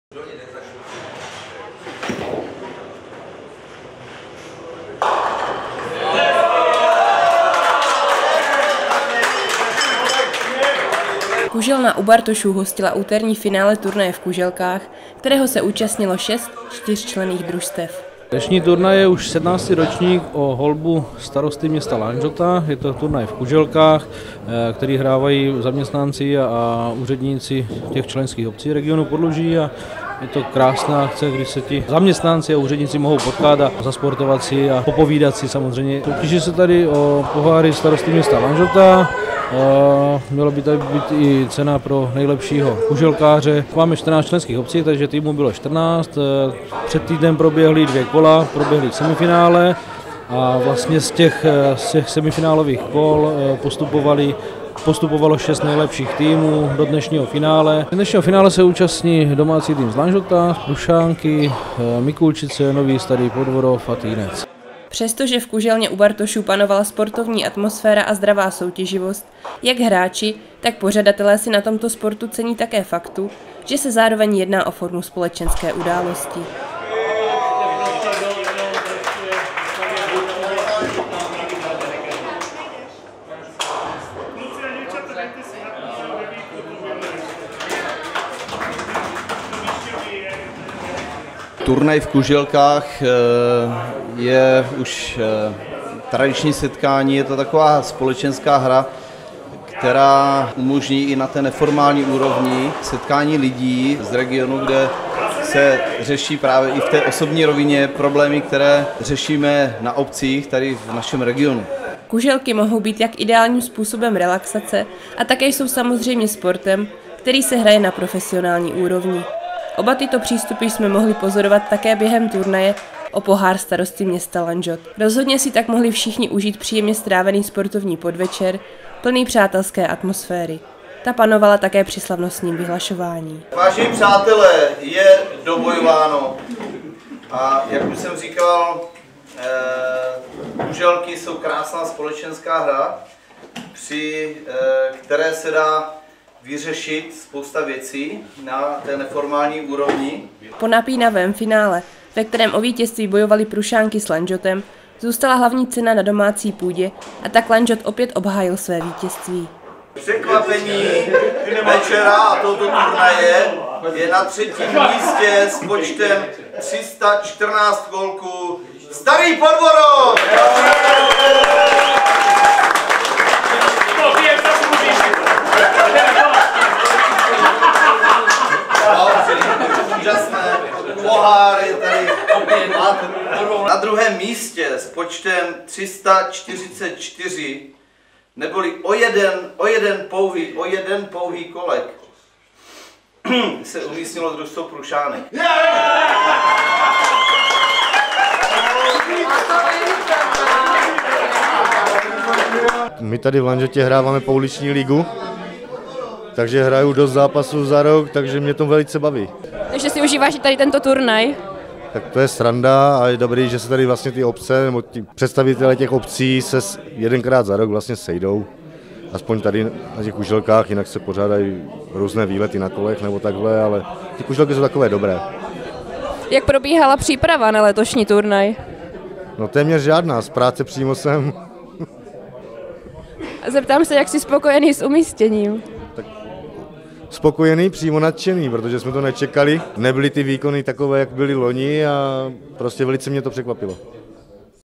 Pohár starosty města Lanžhot zůstal na domácí půdě
Na domácí půdě zůstal Pohár starosty města Lanžhot, o který již po 17. soutěžily týmy členských obcí Regionu Podluží. Kuželna U Bartošů hostila úterní finále turnaje v kuželkách, kterého se účastnilo 6 čtyřčlenných družstev.